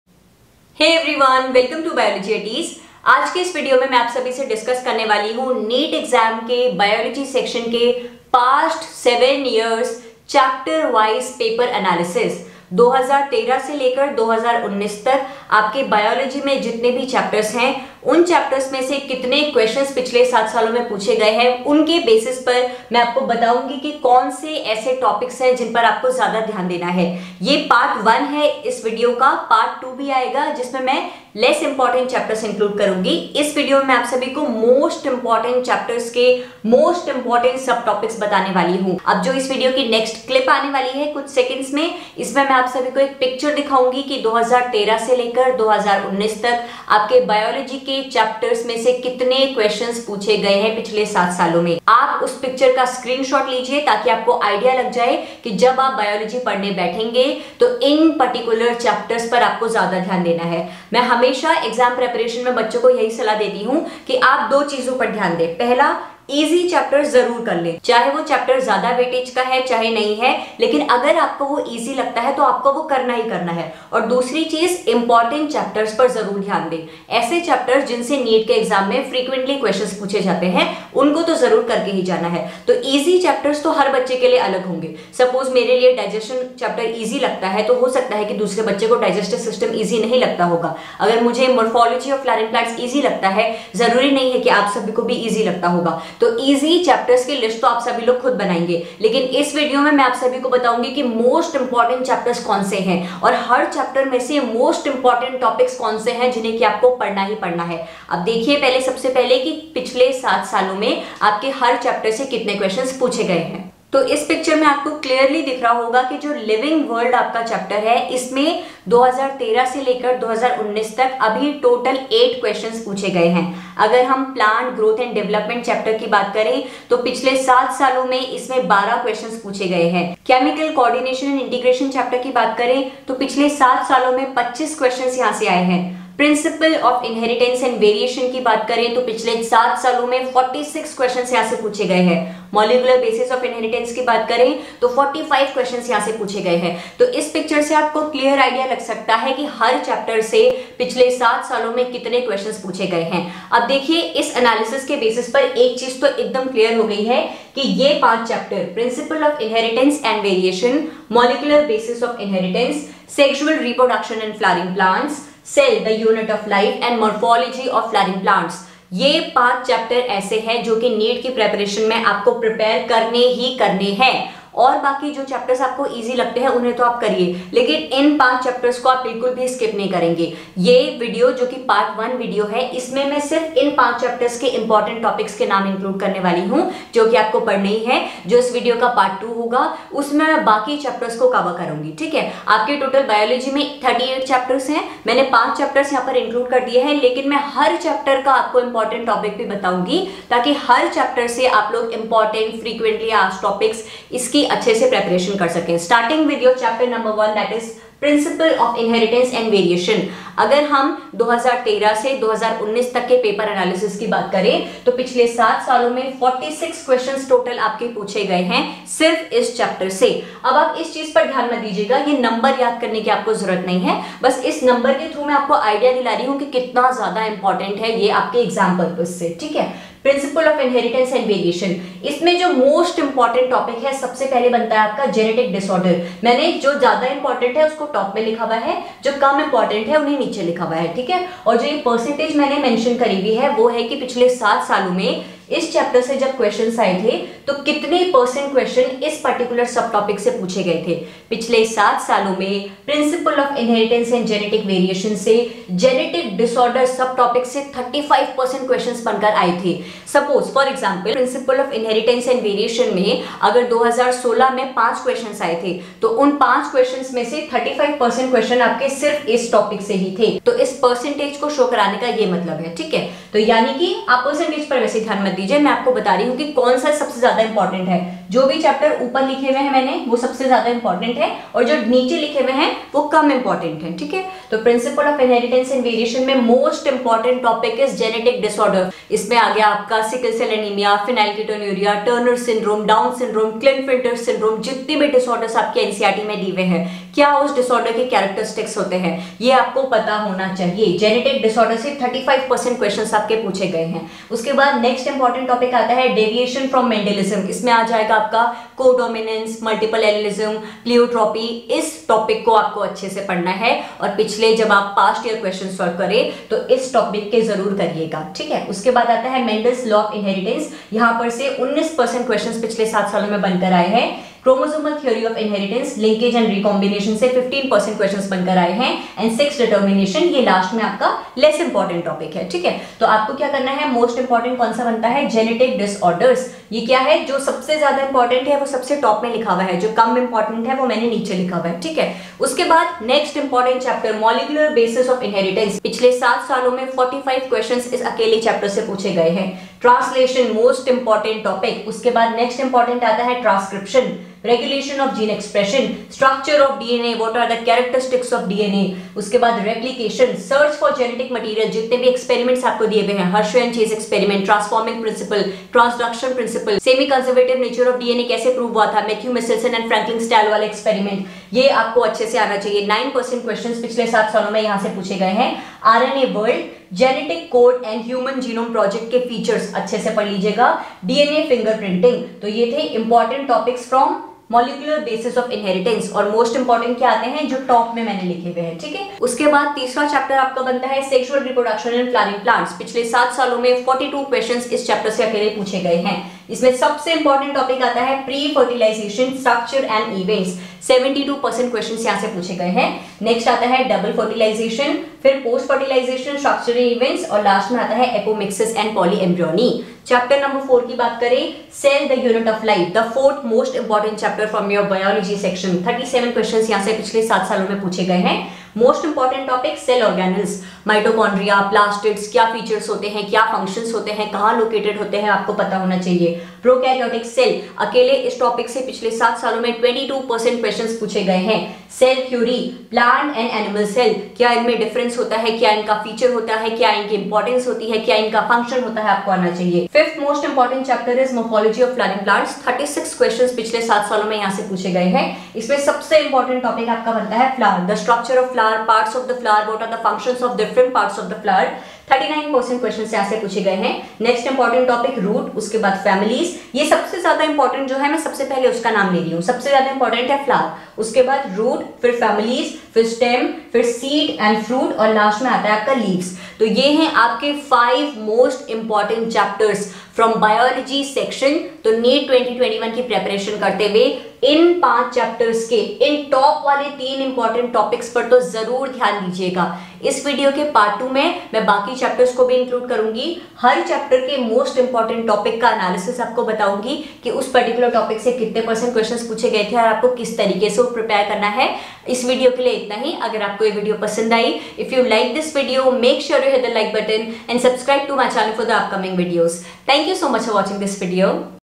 NEET 2021 | BIOLOGY | 5 Most IMPORTANT Chapters | Topic-wise Weightage
In this video I have provided Topic-wise Weightage Analysis of Past 7 years (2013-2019 NEET Biology Chapters.
Do prepare the 5 Most IMPORTANT Chapters and topics mentioned in the video.